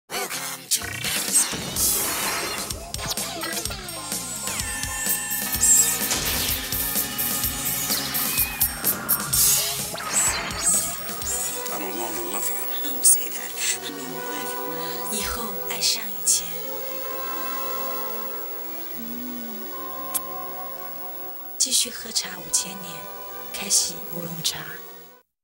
Kaixi Oolong Tea commercial (1999)
This advertisement for oolong tea was found by chance when, after working in practice, I decided to go to Tumblr, which I might return to because I left it earlier because I was very bored, but this is not the point. I found this ad on one of the Tumblr pages about Y2K Aesthetic Institute where there were quite nice looking two-dimensional and three-dimensional arts that were present not only as art, they also added to the covers of magazines, music albums, TV, advertisements and even in games this fashion was used in late 90s to early 2000s. I haven’t seen such a fashion of the 2000s before, but now I’m very interested and it’s cool to look at this art that I could put on my desktop. Well, it seems like he told me everything, but now I will not be busy preparing for exams for a long time, but for now I can afford to lay out random stuff.